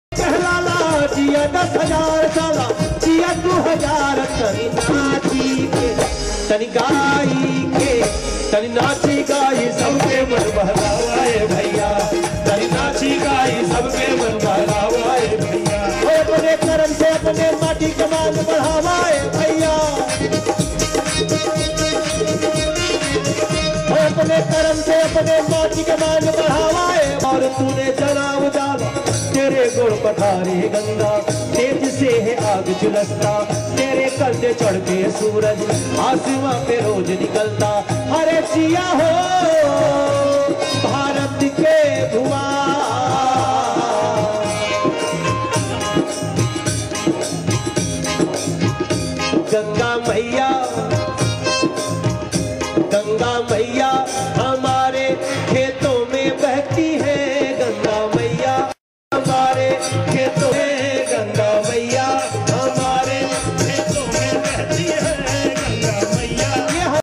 Manoj Tiwari and Nirahua(Dinesh Lal Yadav) performing on CHHAT
MP Manoj Tiwari and Nirahua Performing on Bhojpuri Songs on the Occasion of Chhat Puja at Noida Stadium.